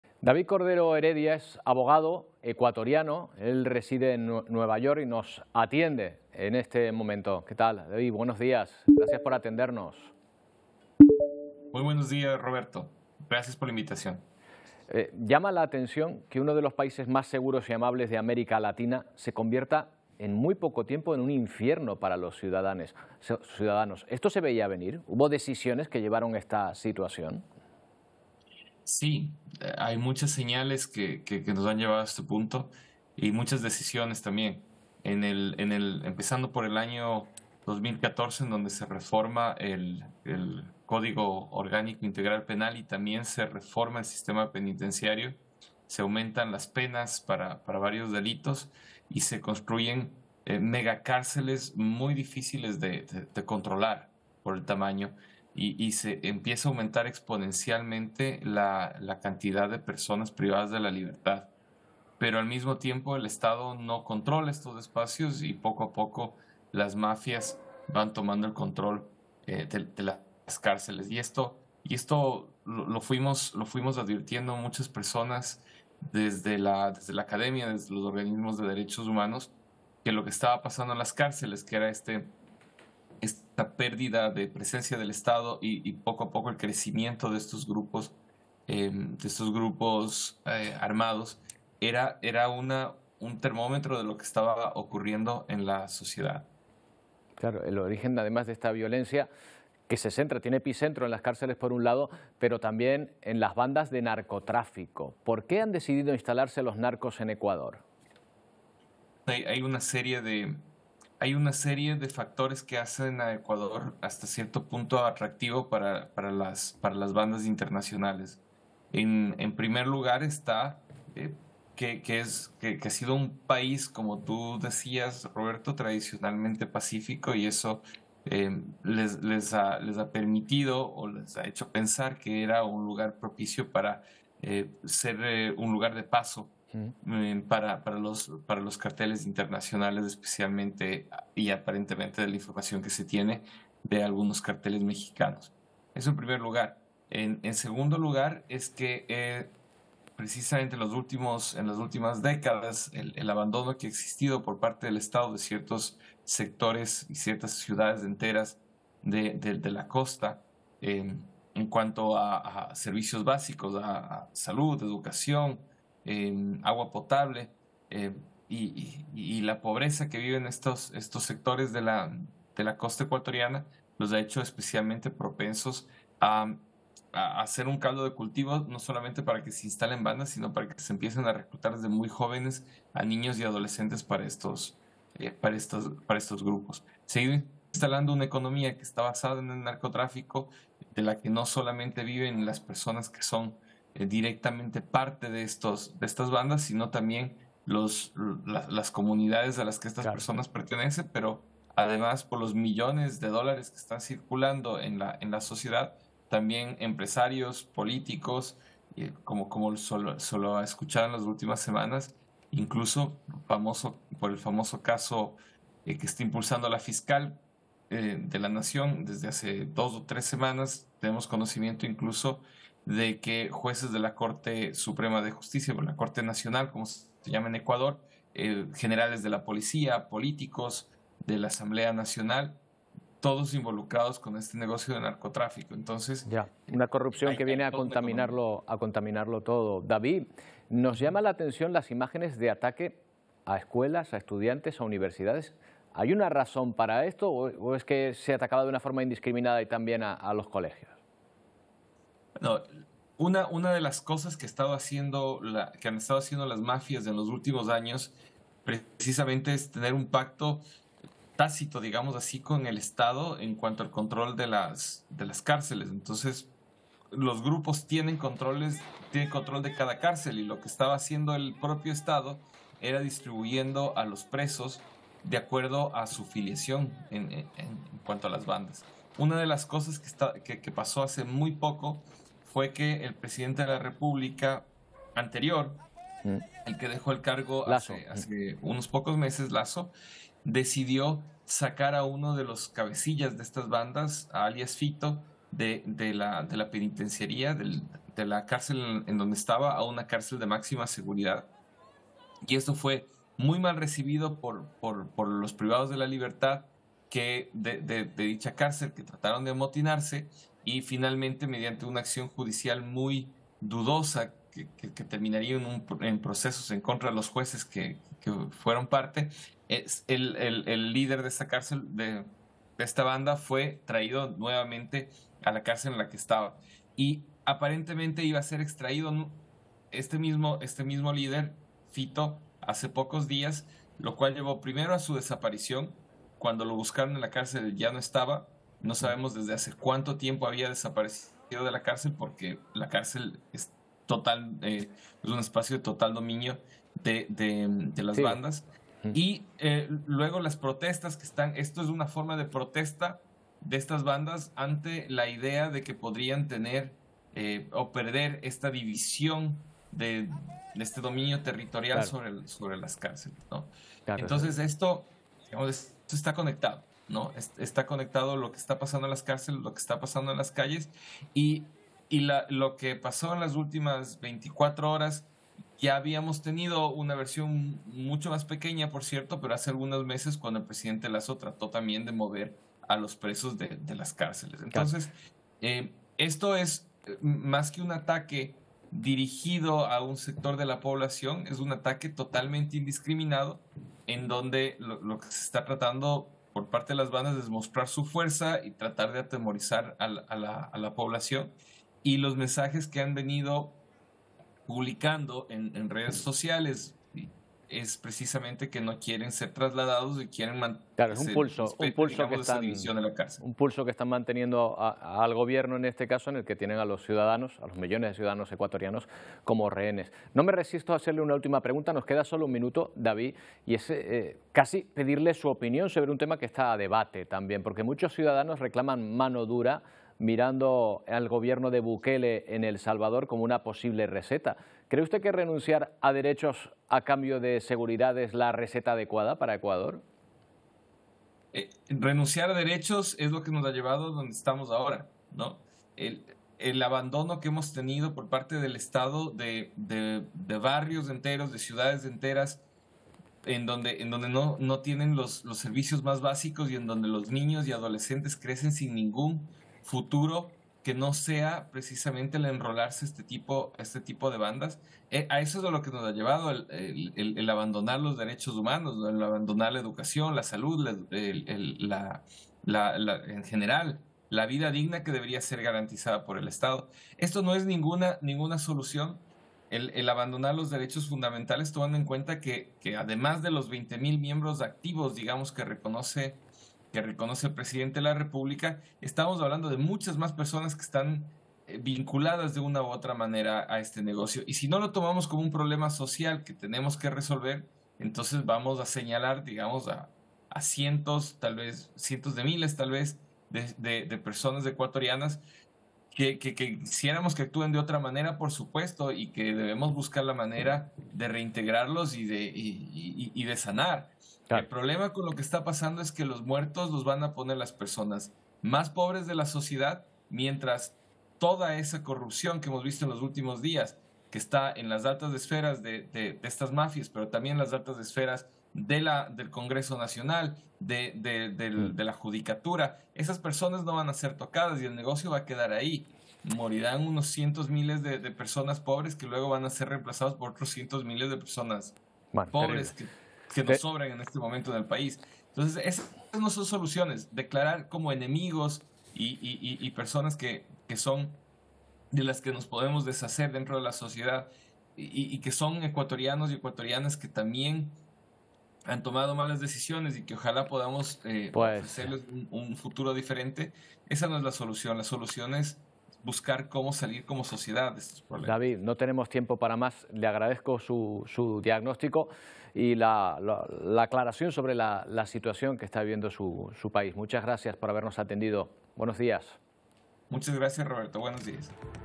Estado de guerra en Ecuador
La población civil ha vaciado las calles de Ecuador estos días, dejando imágenes solo comparables con la época de pandemia y una enorme incertidumbre sobre el futuro. Hablamos con un abogado ecuatoriano en Nueva York que nos da viarias claves sobre la situación que se vive en su país.